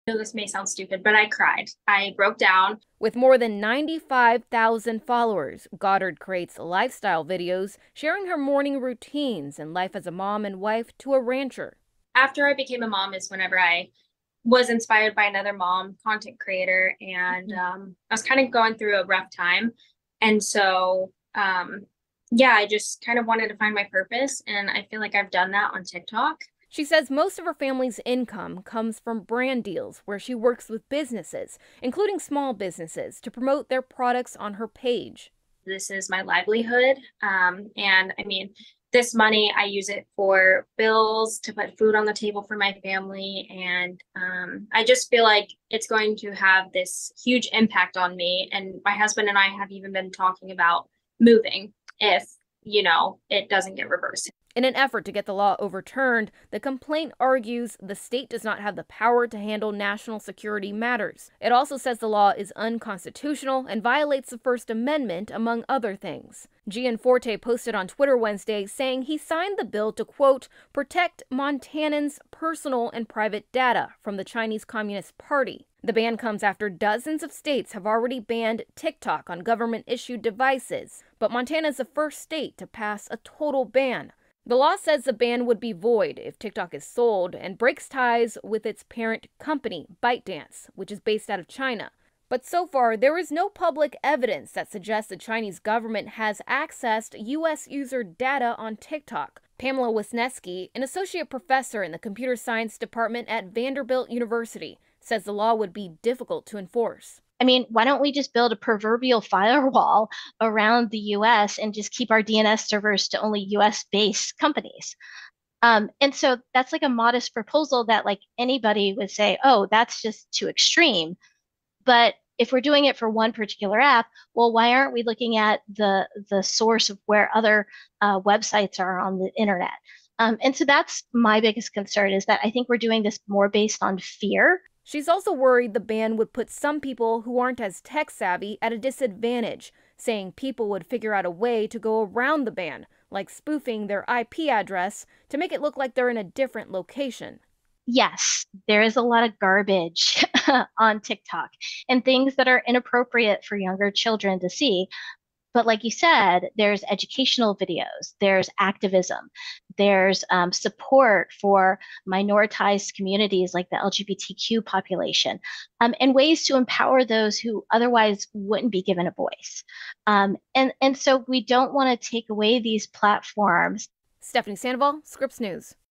TikTok creator suing Montana says the app is her 'livelihood'
Five TikTok content creators are suing the state of Montana after the state’s governor, Greg Gianforte, signed a bill into law that would ban the app.  i